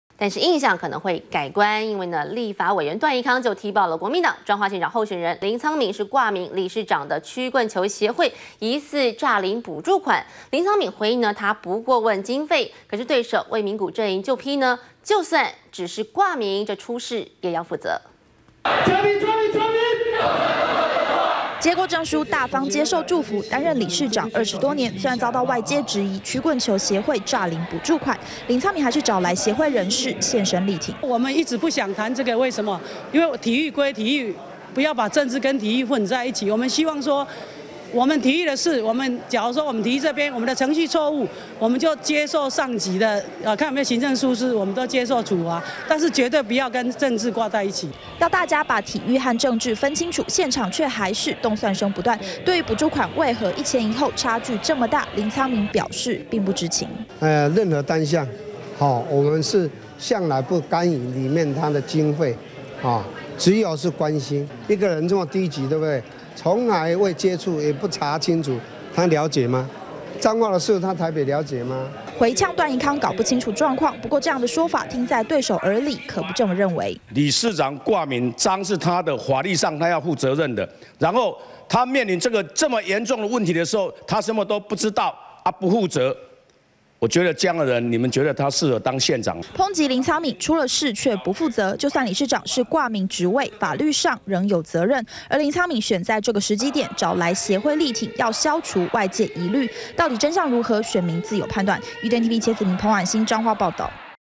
【2014.11.16】曲棍球協會涉弊 林滄敏:不管經費 -udn tv
立法委員段宜康踢爆，國民黨彰化縣長候選人林滄敏掛名理事長的曲棍球協會，疑似詐領補助款，林滄敏回應他不過問經費，但對手魏明谷陣營就批評，就算只是掛名，出事也要負責。完整內容 , FB , Twitter , G+